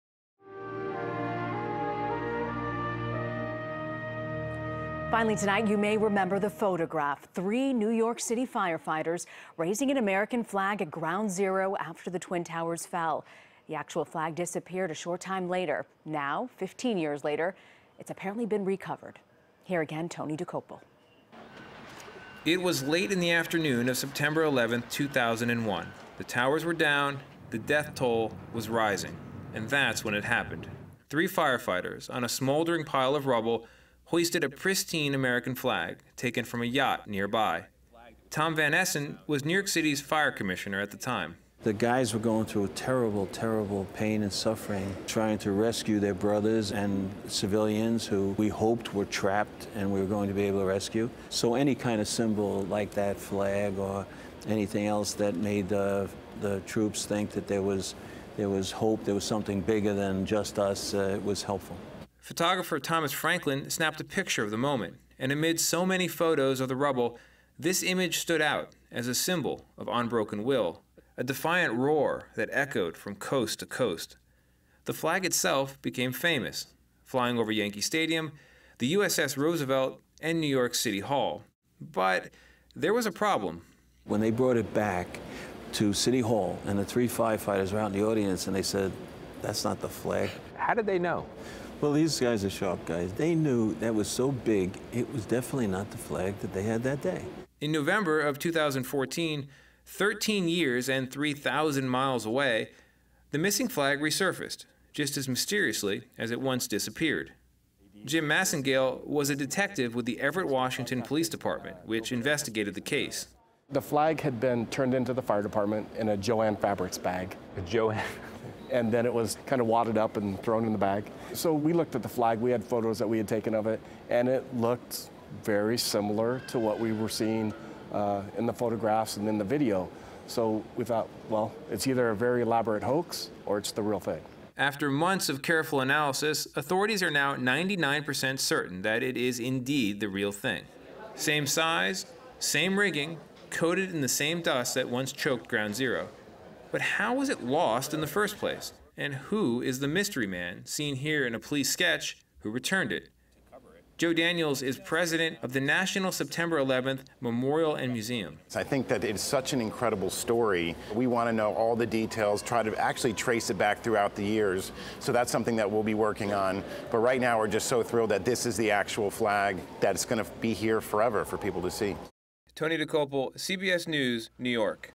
"Ground Zero Flag" returns home
In an iconic photo, three firefighters raised an American flag in the rubble of the World Trade Center after the 9/11 attacks. The flag later disappeared, but it's recently been returned to ground zero. Tony Dokoupil has more.